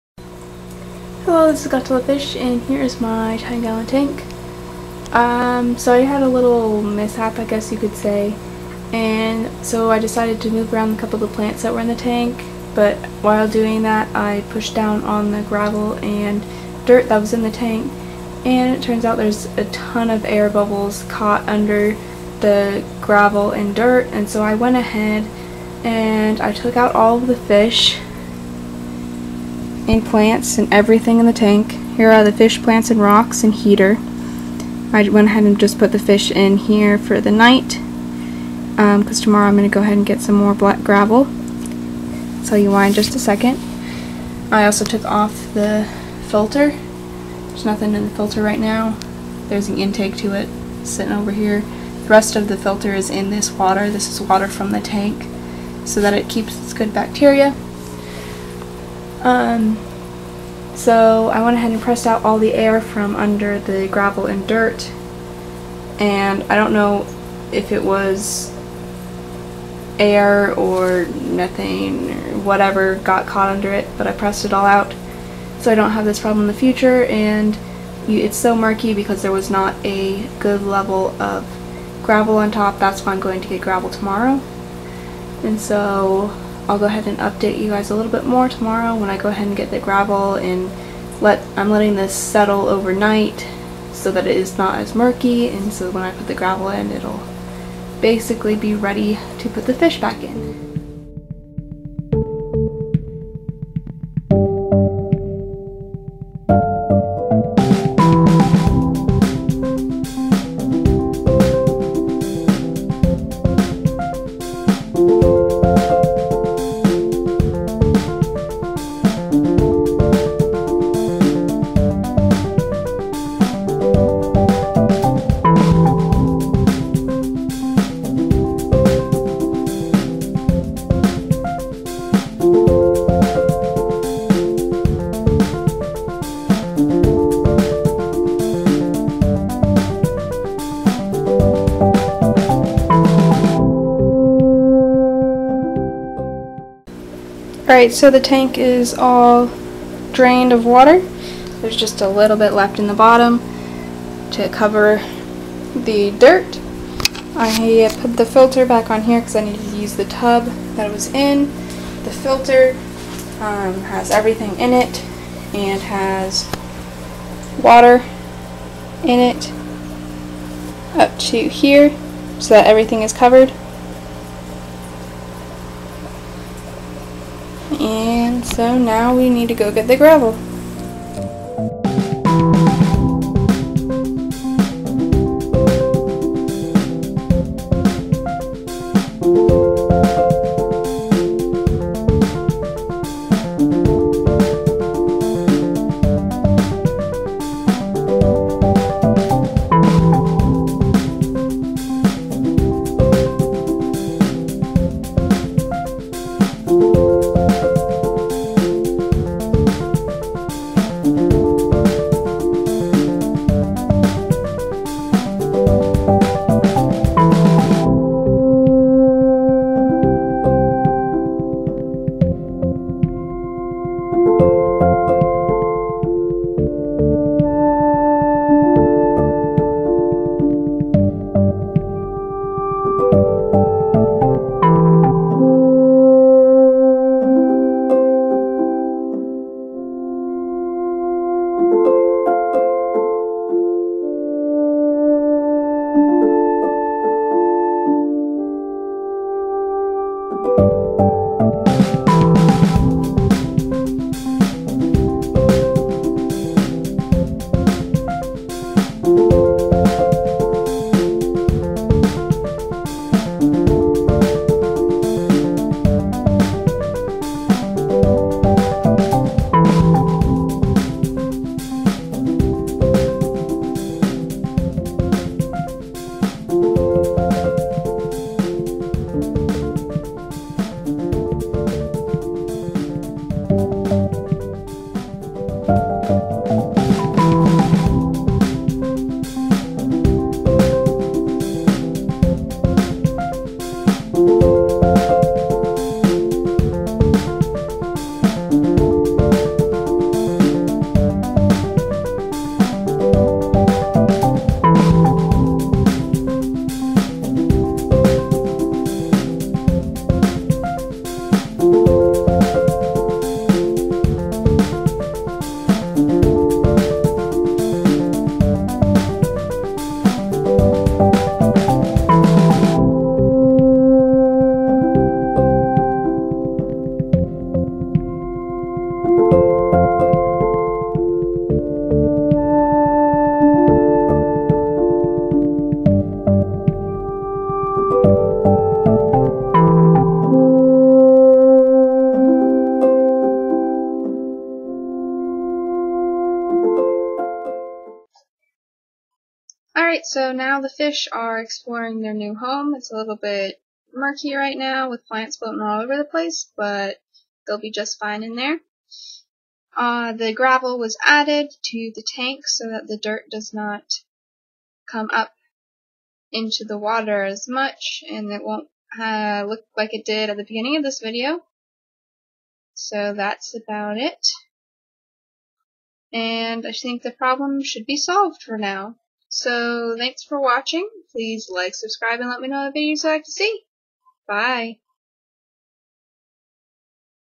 Air Trapped Under Substrate, Lets Fix It!
My 10 gallon tank got a bunch of air trapped in the substrate so I had to press it out which caused this mess, watch to see how I got the tank clean again!